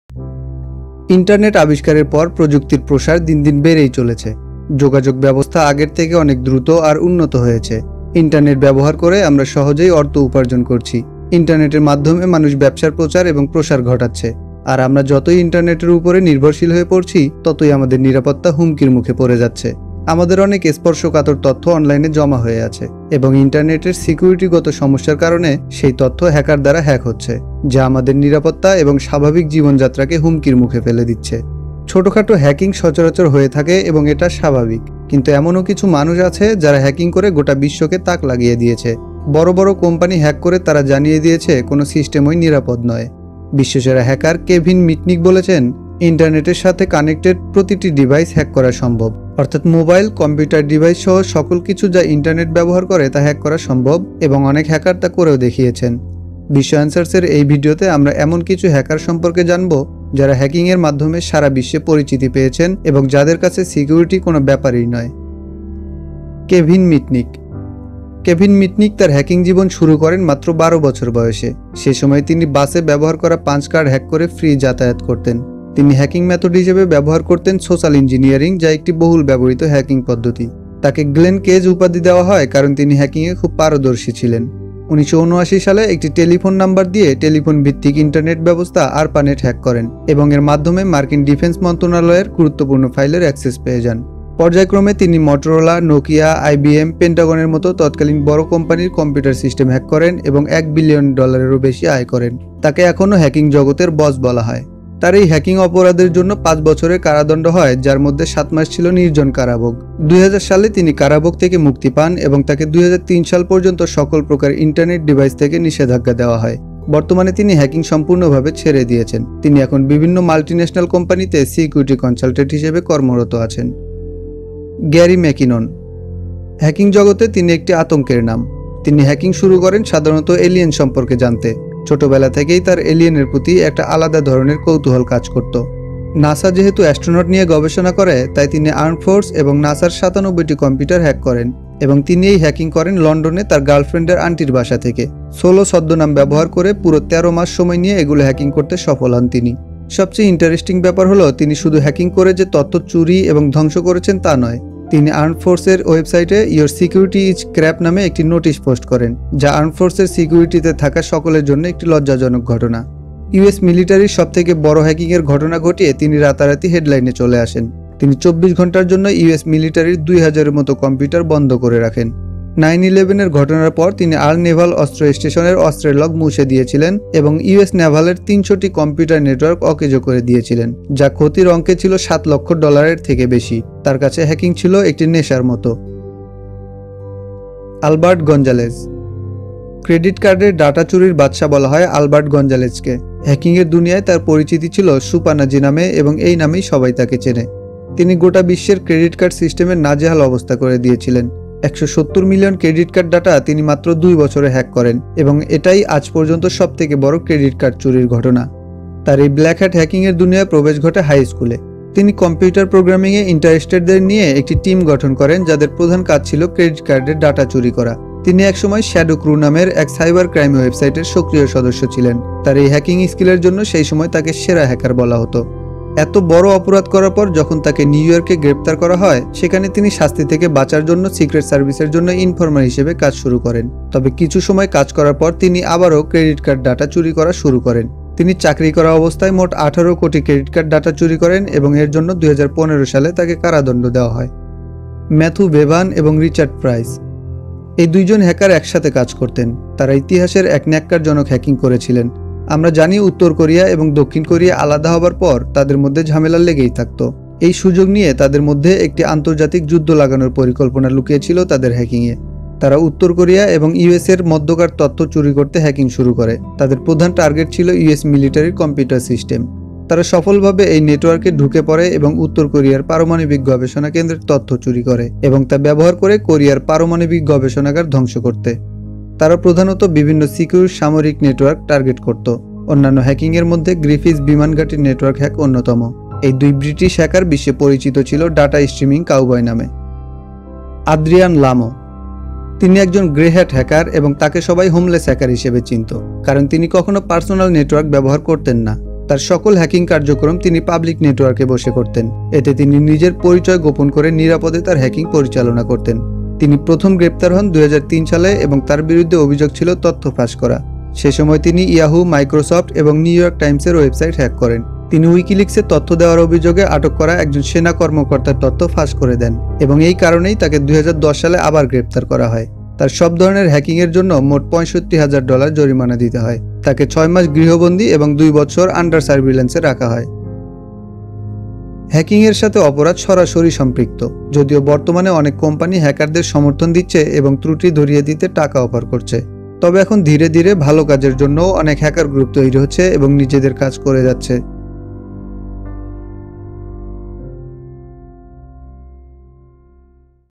বিশ্বসেরা ৫ জন হ্যাকারের জীবনী এবং কর্মকাণ্ড || 5 Most Dangerous & Famous Hackers.
ছোট খাটো হ্যাকিং সচরাচর হয়ে থাকে এবং এটা স্বাভাবিক। কিন্তু এমনও কিছু মানুষ আছে যারা হ্যাকিং করে গোটা বিশ্বকে তাক লাগিয়ে দিয়েছে। বড় বড় কোম্পানি হ্যাক করে তারা জানিয়ে দিয়েছে কোন সিস্টেমই নিরাপদ না। বিশ্বসেরা হ্যাকার কেভিন মিটনিক বলেছেন “ইন্টারনেটের সাথে কানেক্টেড প্রতিটি ডিভাইস হ্যাক করা সম্ভব”। অর্থাৎ মোবাইল, কম্পিউটার ডিভাইস সহ সকল কিছু যা ইন্টারনেট ব্যবহার করে তা হ্যাক করা সম্ভব এবং অনেক হ্যাকার তা করেও দেখিয়েছেন। বিস্ময় অ্যানসারসের এই ভিডিওতে আমরা এমন কিছু হ্যাকার সম্পর্কে জানবো যারা হ্যাকিং এর মাধ্যমে সারা বিশ্বে পরিচিতি পেয়েছেন এবং যাদের কাছে সিকিউরিটি কোন ব্যাপার না।

Video Sections:
0:00​ - Introduction
1:05​ - Kevin Mitnick
2:04​ - Gary McKinnon
3:06 - Albert Gonzalez
4:04 - Mathew Bevan and Richard Pryce
4:50 - Adrian Lamo
5:30 - The End